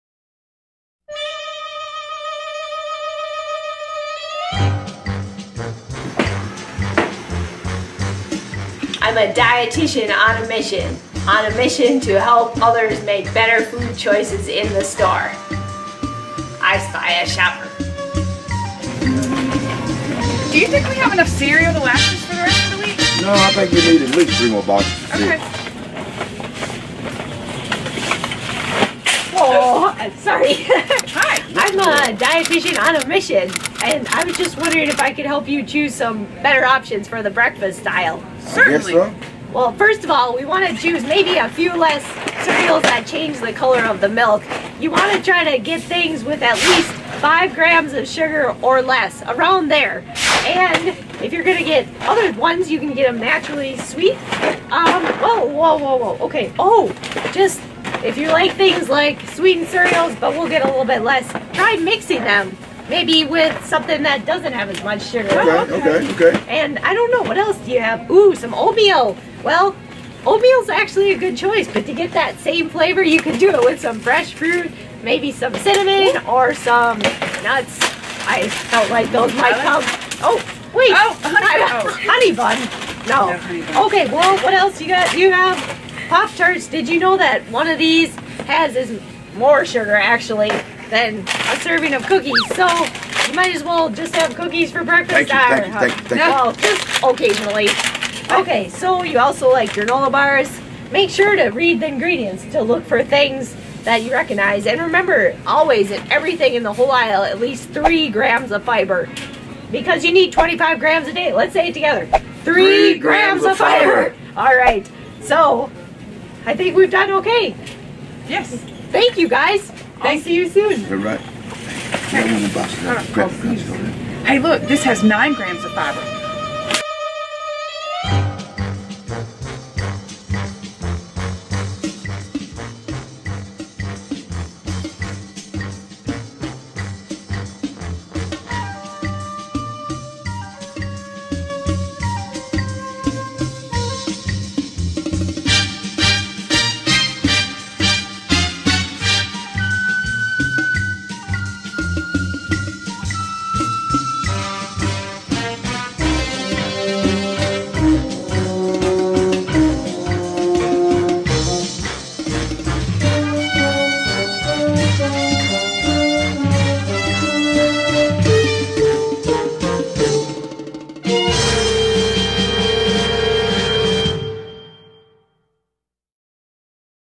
Dietitian on a Mission - Cereal
The Dietitian on a Mission is spotted showing shoppers how to make smart choices when selecting cereals.  Watch out, she may be on a mission at a grocery store near you, soon!